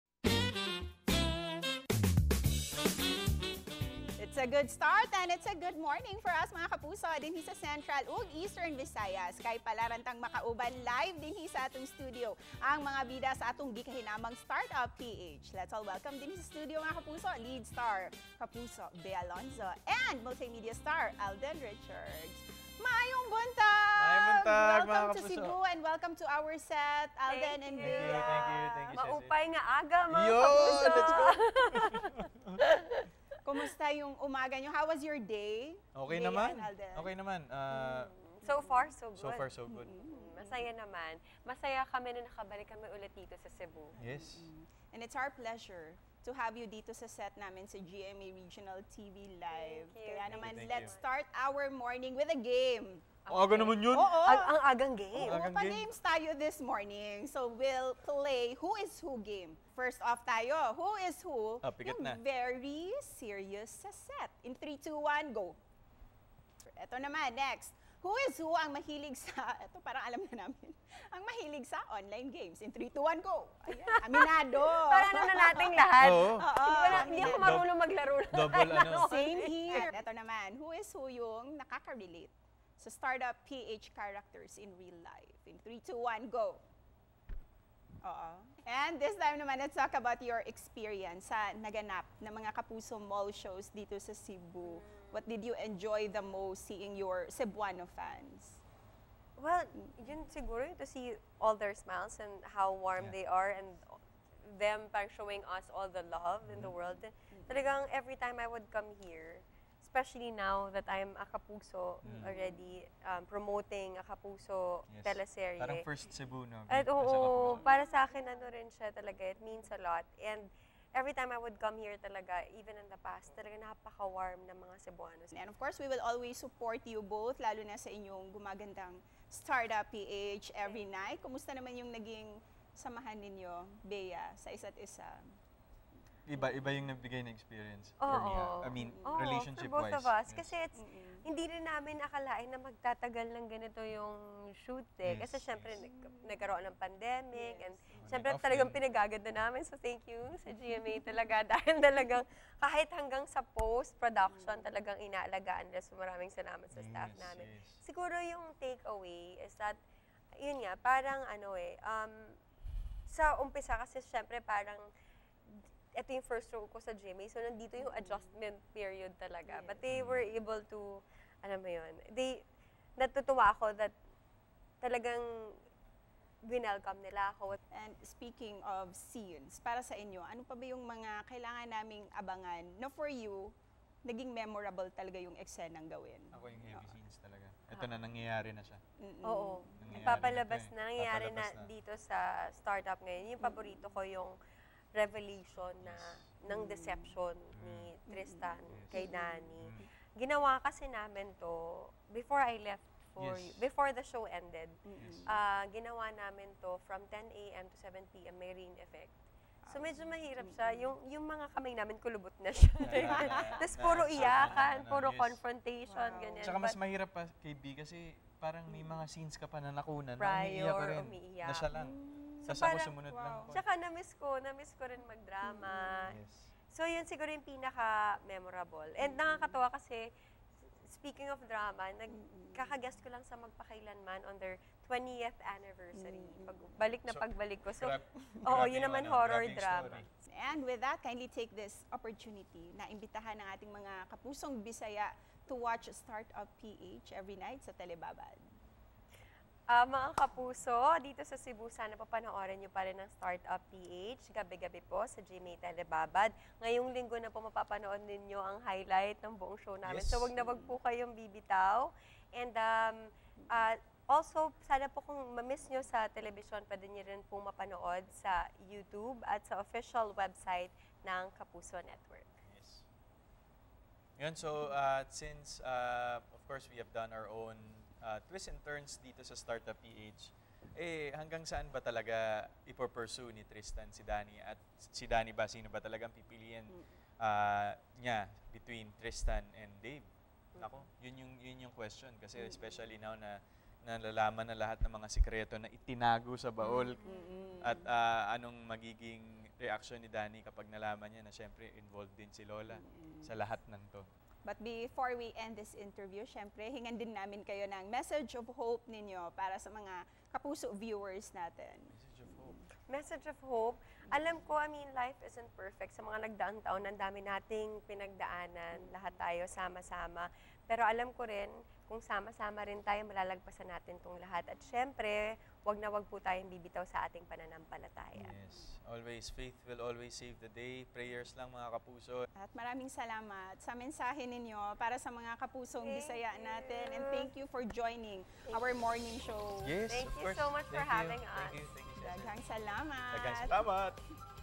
GMA Regional TV Live: ‘Start-Up PH’ Lead Stars sa Cebu
‘Start-Up PH’ lead stars, Alden Richards ug Bea Alonzo, malipayong nakita pagbalik ang mga Kapusong Cebuano sa duha ka mga mall show. Mapasalamaton sila sa ilang supporters nga walay puas nga nagtan-aw sa ila’ng labing unang Kapuso project sa usa'g usa.